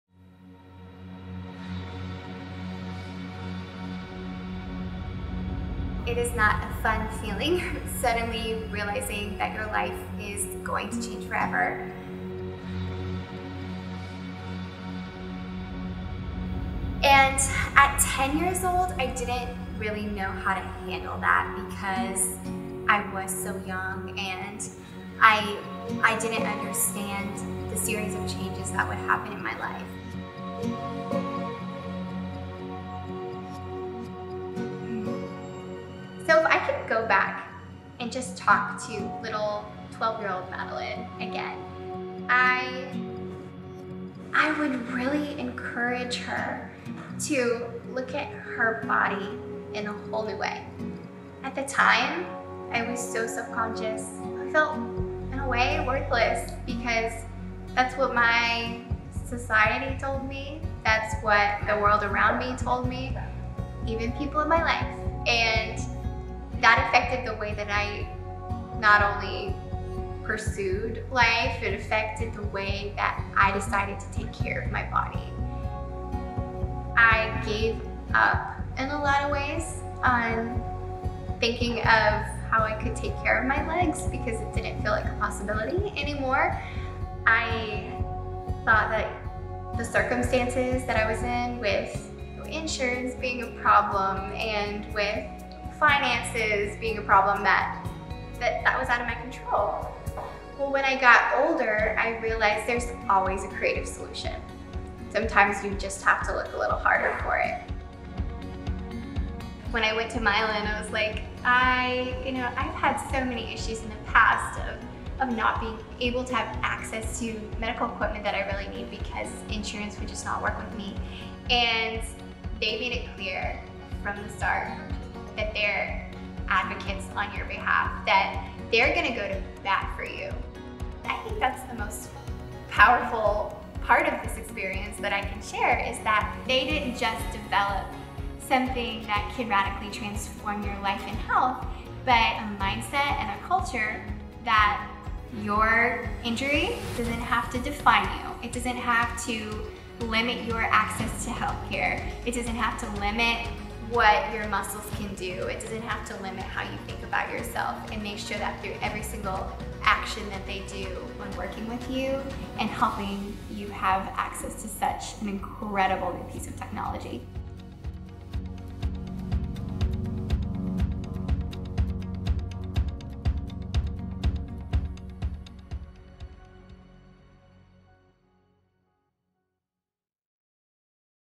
Meet Madeline Delp | MYOLYN
MYOLYN's brand ambassador Madeline Delp discusses her rehabilitation journey and how the MyoCycle empowers her to move. MYOLYN uses functional electrical stimulation (FES) to help users achieve therapeutic exercise and functional outcomes, despite spastic muscles, spasticity, muscle weakness or paralysis caused by disorders like spinal cord injury, multiple sclerosis, and stroke. FES works by using small amounts of electrical stimulation to help muscles contract and produce functional movements.